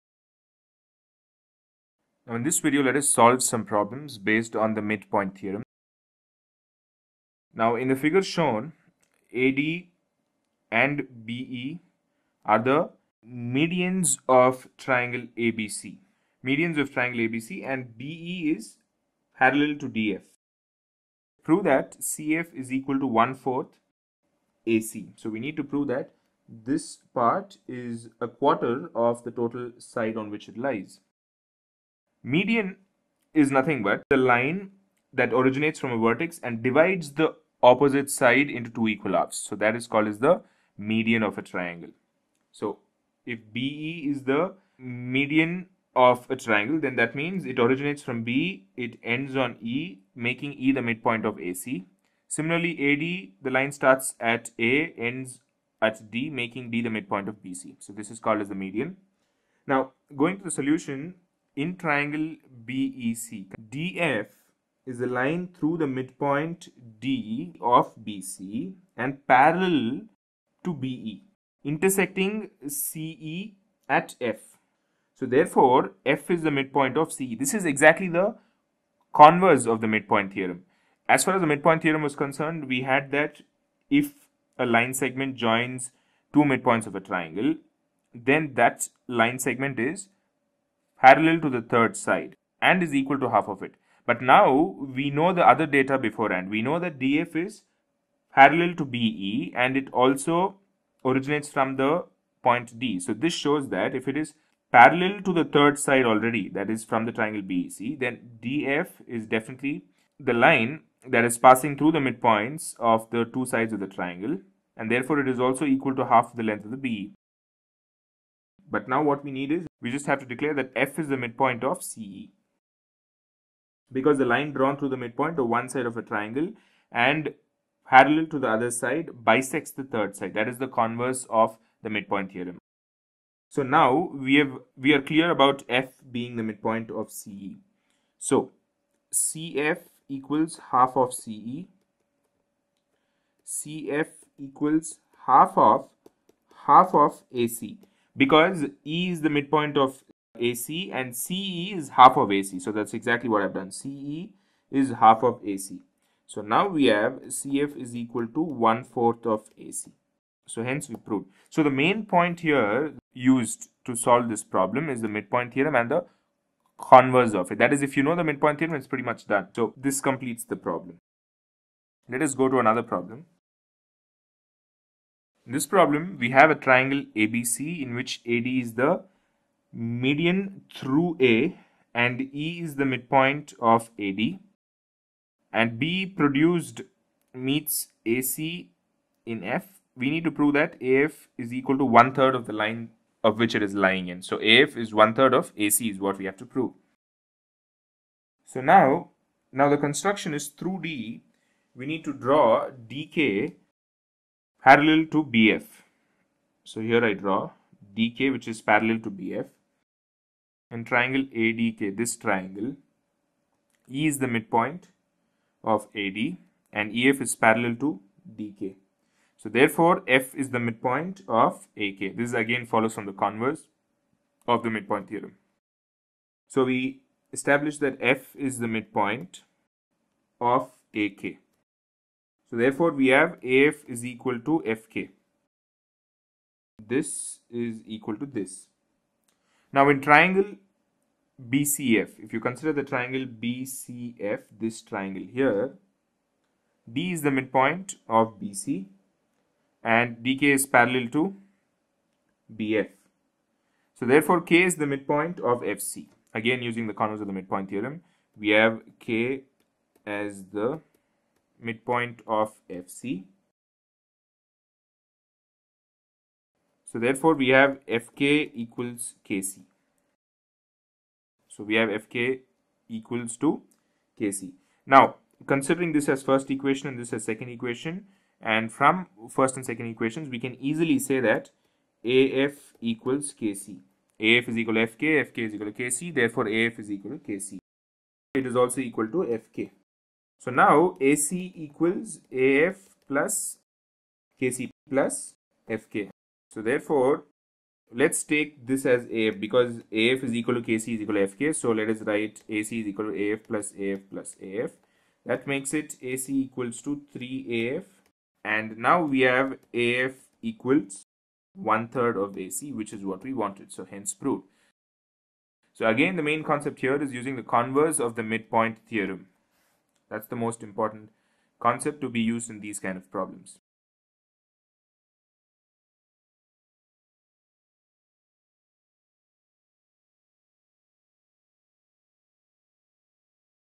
How to solve problems on mid-point theorem ?
Learn how to solve problems based on midpoint theorem by watching this video.
Do watch the video on midpoint theorem to understand this video.